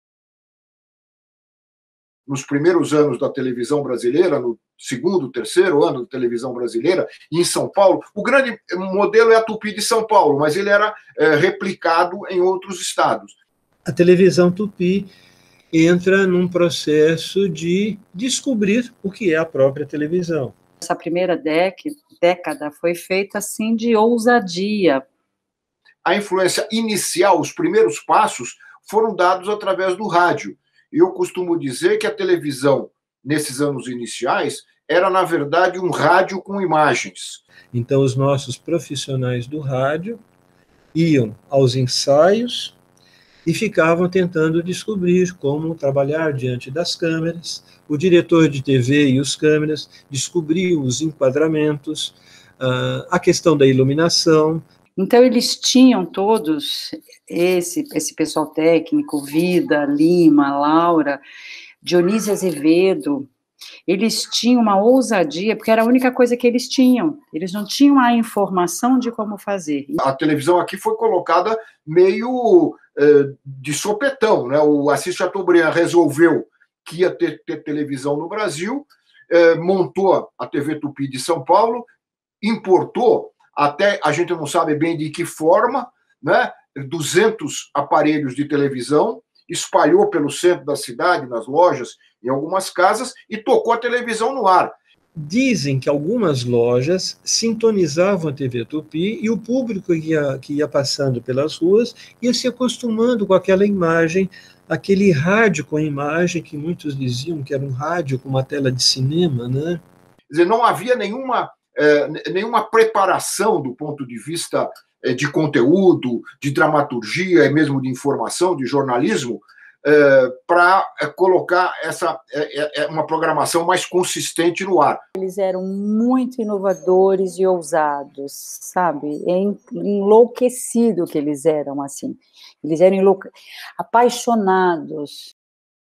70 anos da TV no Brasil: A influência do rádio
Pesquisadores explicam influência do rádio para o início da TV
Segundo eles, a TV, há 70 anos, era uma rádio com imagens. Confira mais na reportagem da Agência Brasil

Edição e reportagem:
Leyberson Pedrosa
Luiz Cláudio Ferreira

Coordenação: Bia Arcoverde